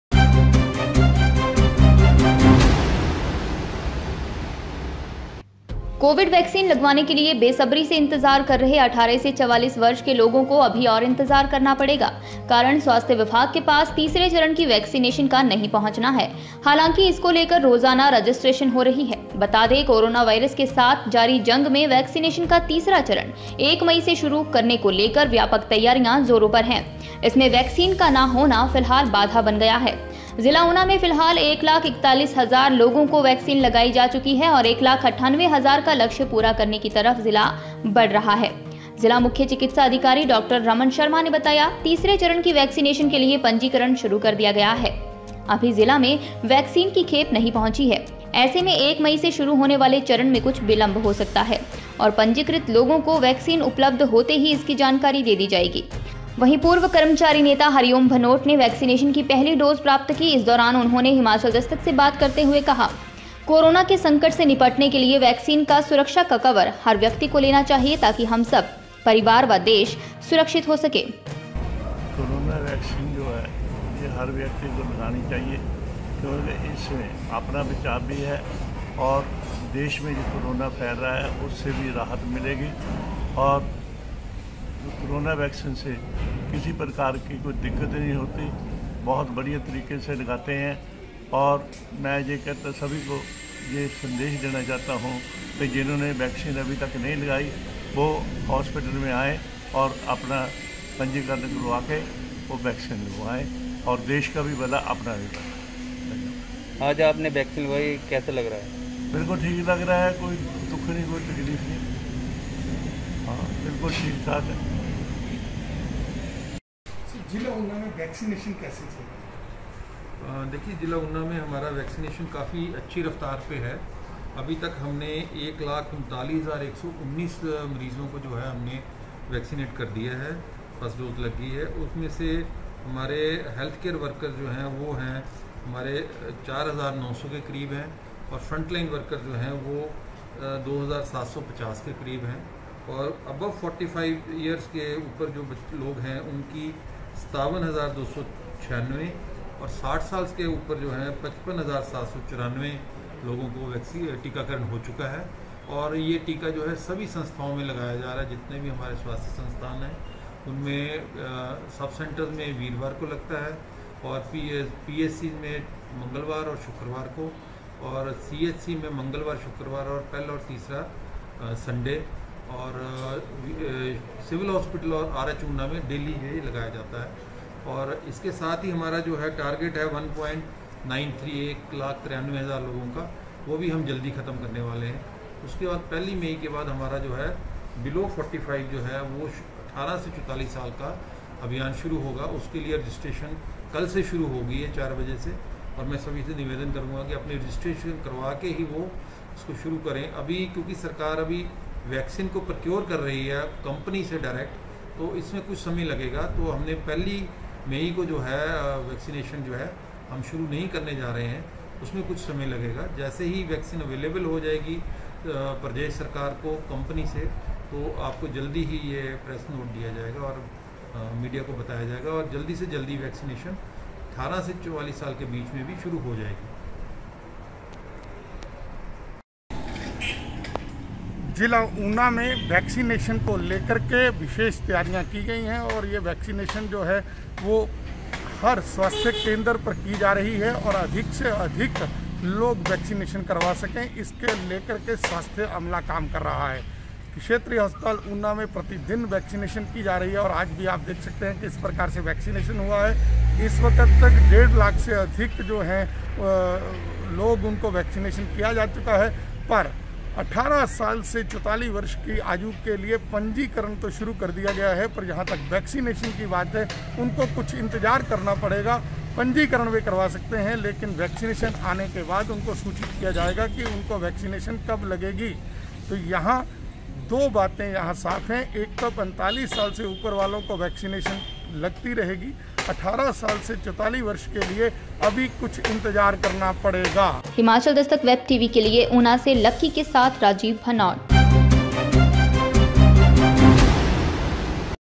18 से 44 वर्ष के लोगों को वैक्सीन के लिए करना होगा इंतजार
18 से 44 वर्ष के लोगों को वैक्सीन के लिए करना होगा इंतजार